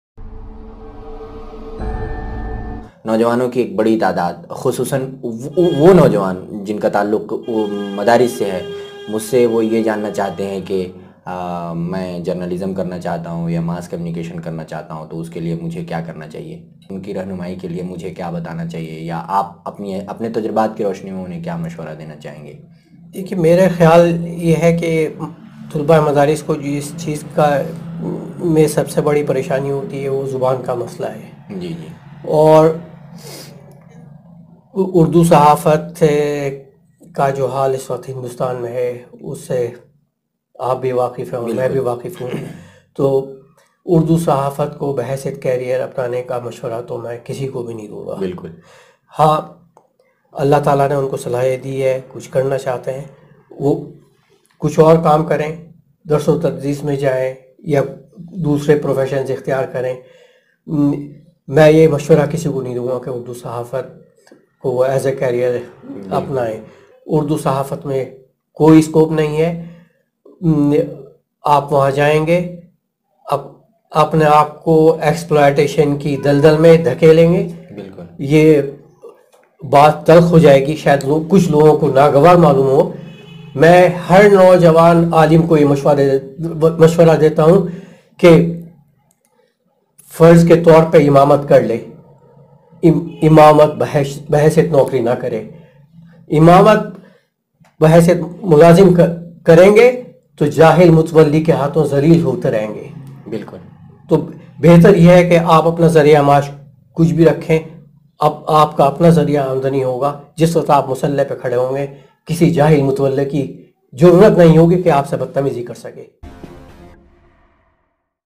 سینئر جرنلسٹ کی صلاح مسلم نوجوان اردو صحافت اور مسجدوں کی امامت کو بطور کریر نہ اپنا ئیں
Asia Times TV
 ناظرین پہلے مکمل انٹرویو دیکھیں پھر اپنا کمینٹ کریں،  درج ذیل لنک  کلک کریں